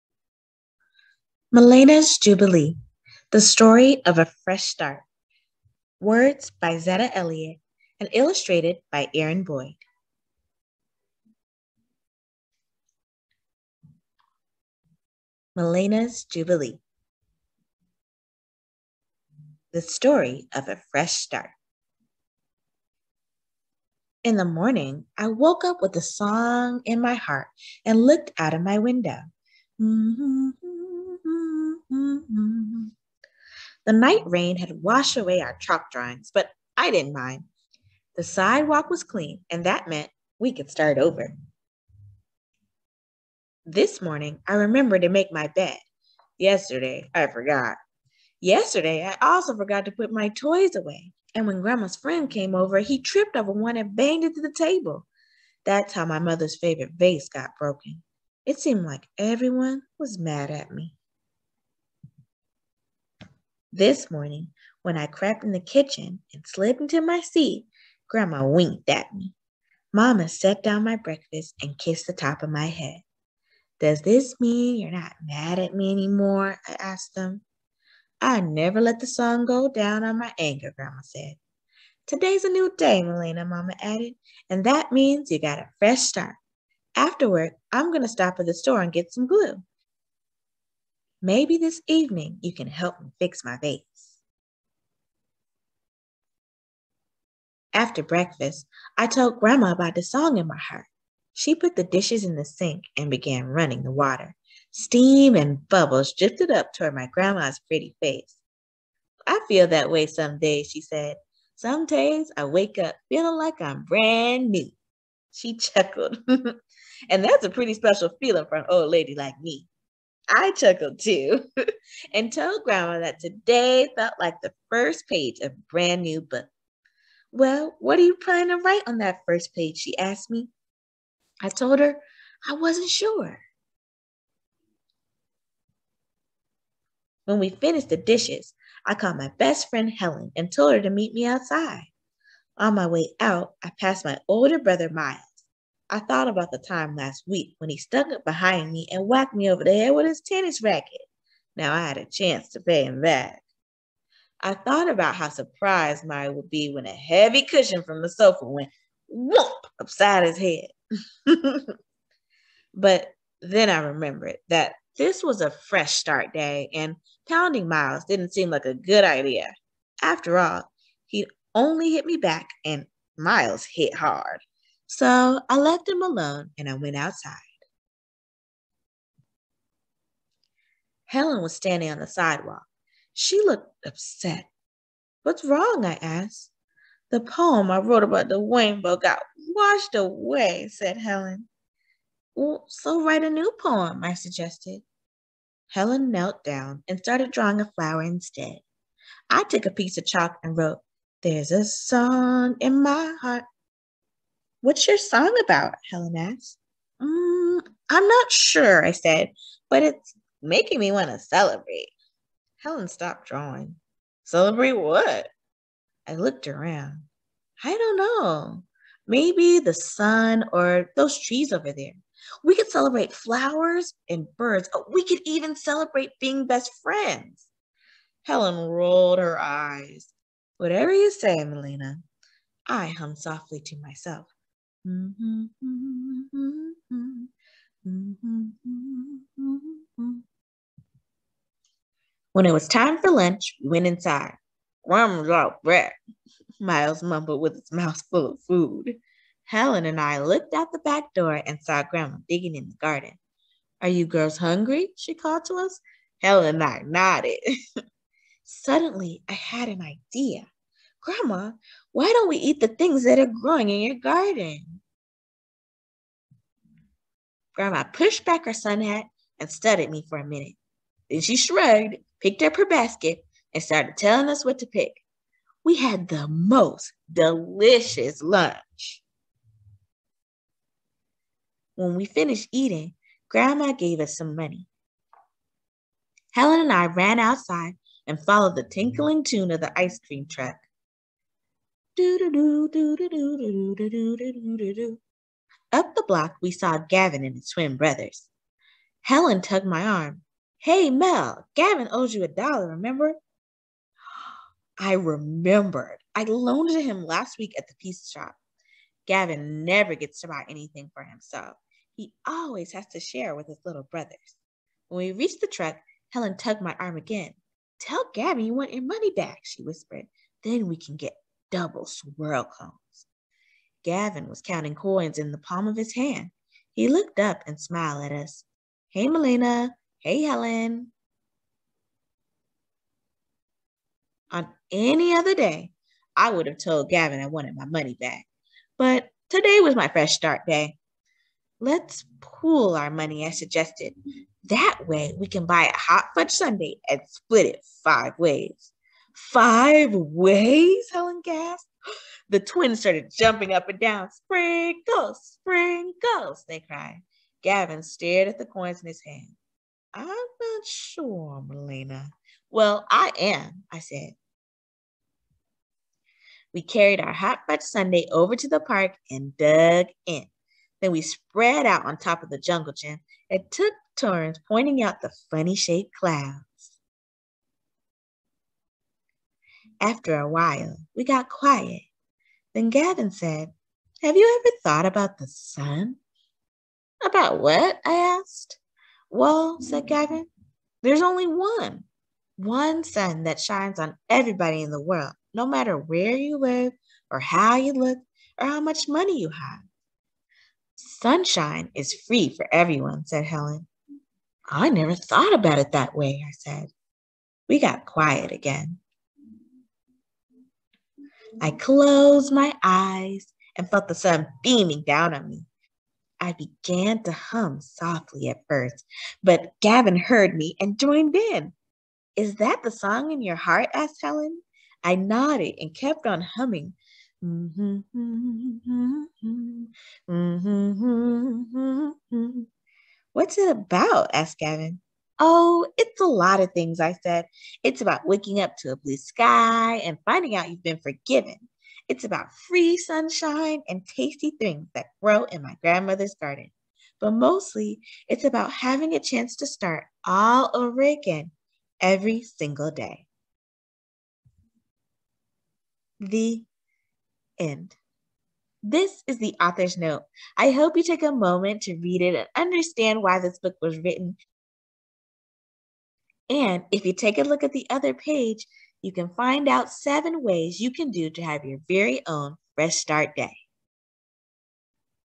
Melena's Jubilee | Kids Read Aloud Books | Black History Month Read Aloud | Classroom Read Alouds
Forgiveness makes the heart grow stronger! Melena made some mistakes yesterday but today is a new day and she is excited to live her best life! Come with her and her friends as they discover the beauty of a fresh start.

Today's Book is: Melena's Jubilee
Author: Zetta Elliot
Illustrator: Aaron Boyd

Please purchase the book and support the author! Further your child's education with a great story and think about Supporting a Black Owned Business while you do it!